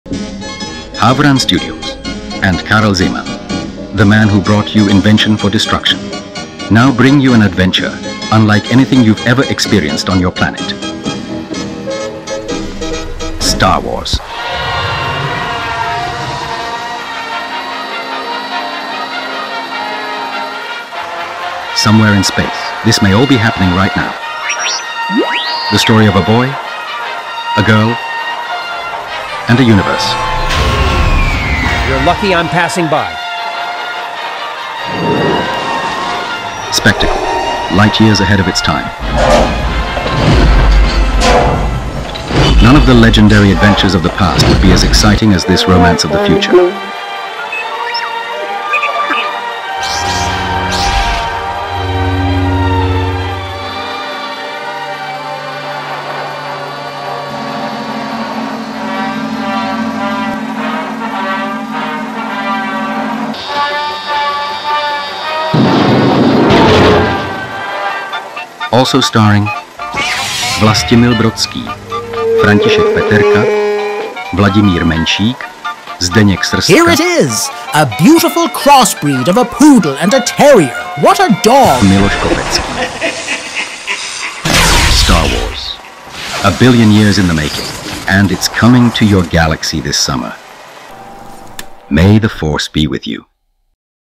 What if Karel Zeman was filming Star Wars? AI trailer - english version.
One sometimes wonders what some films would look like in an alternative world. What if, for example, Quentin Tarantino had made The Flint and the Frog? Or if Star Wars was directed by Karel Zeman... oh wait, that would be worth a shot. Still, I wanted to try something more artistically stylized as part of my experiments with AI video and shot consistency. And the ageless styling of Karel Zeman sounded like quite a challenge :)

This fictional piece was created as a tribute to both dudes. Both Karel Zeman and George Lucas have influenced generations of filmmakers, and I myself grew up on their work.  Karel Zeman is a Czech director who has inspired many filmmakers including Tim Burton and Terry Gilliam.

The trailer was created using the following AI tools.